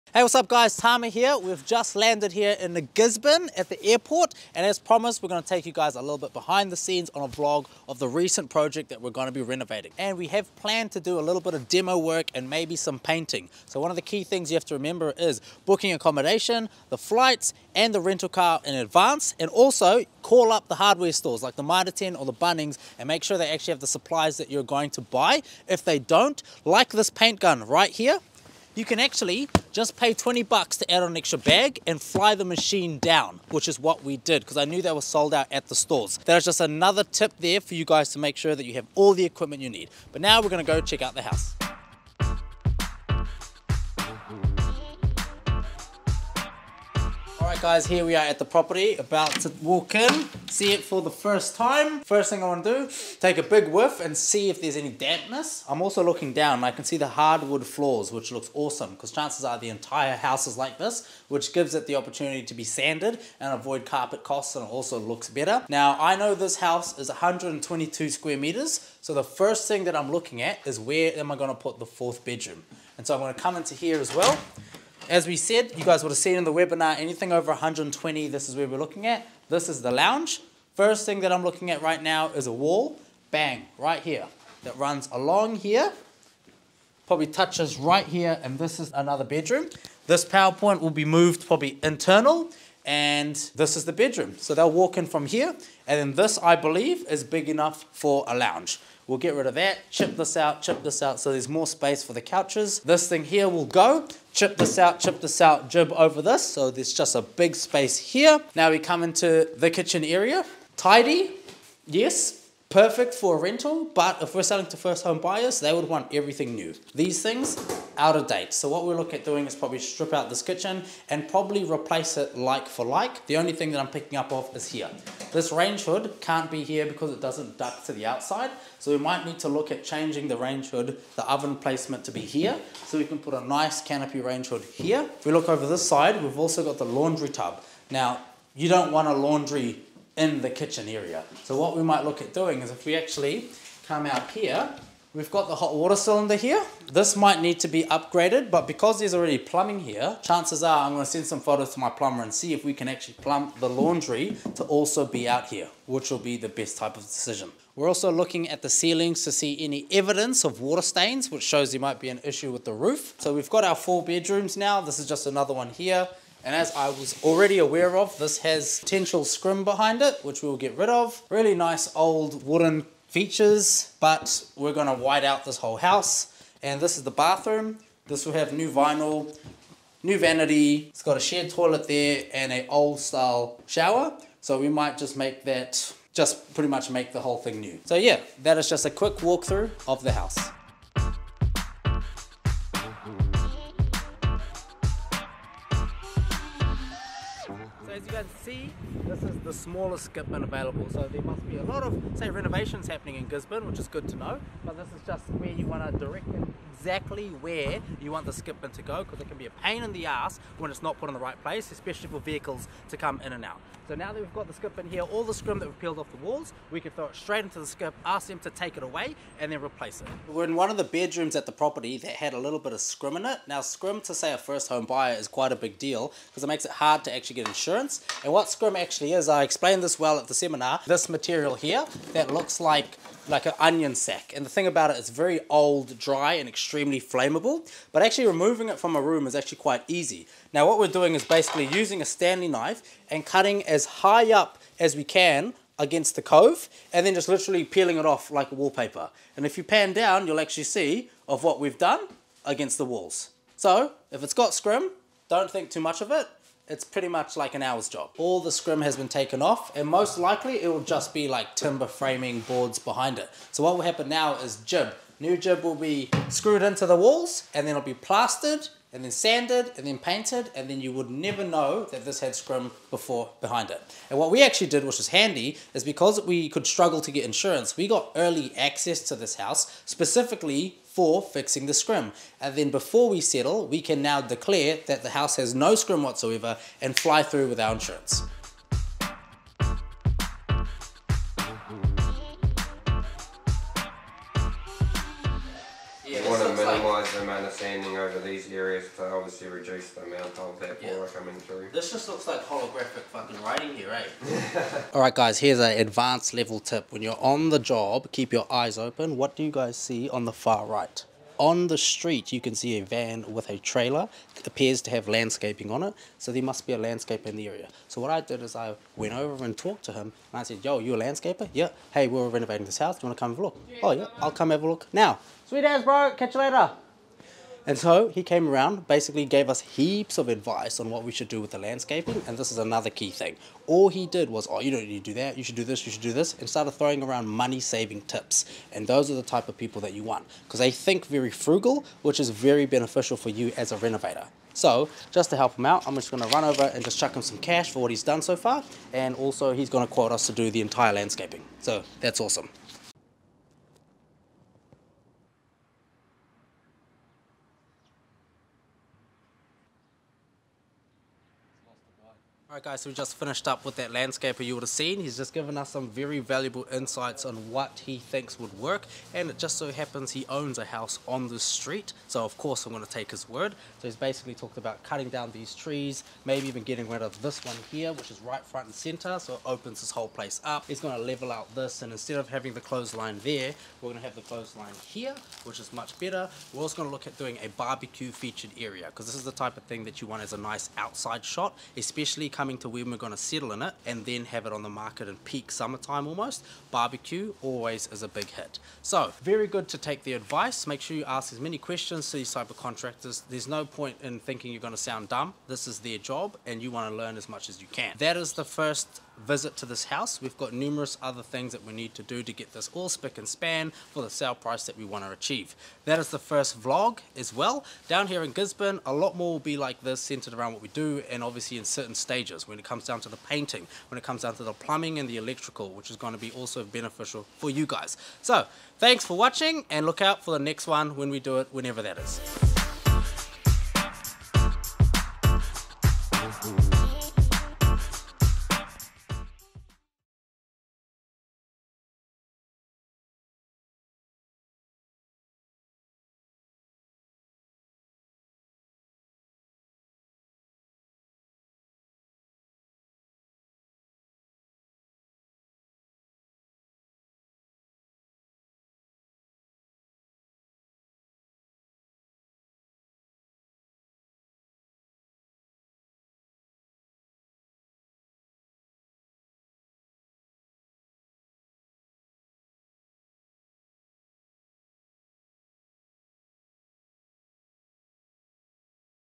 Property Investor explains first steps for renovation including scrim removal and additional bedroom
In my first vlog regarding property, we show what items need to be planned in advance as we travel to Gisborne. Many older houses also contain scrim which could be an issue for insurance, so we break down how to remove it very easily and how we plan on adding another bedroom to increase the property's cashflow and value. @Tamasingh on Instagram